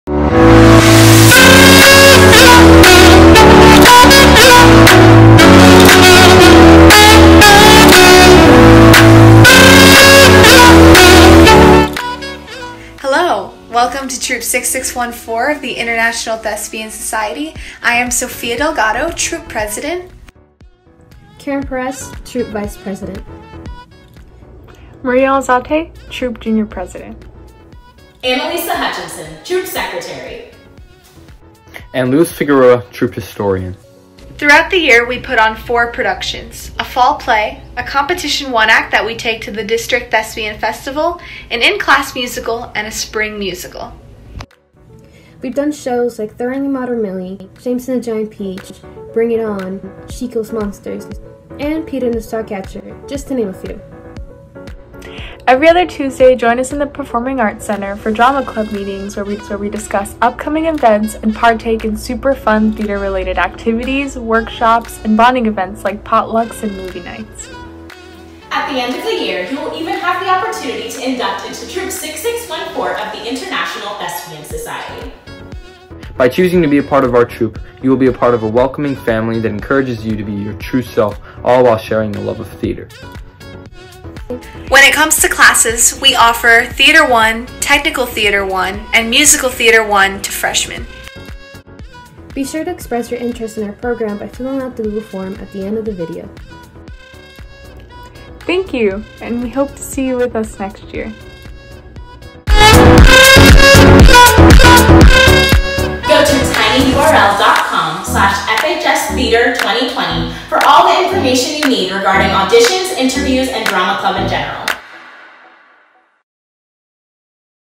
Welcome to the Theatre Department! | Freedom High School Freshmen Orientation 2020
Welcome Class of 2025! Thank you for expressing interest in our phenomenal theatre department here at Freedom High School.

Theatre 1/Musical Theatre 1

Theatre 1/Technical Theatre 1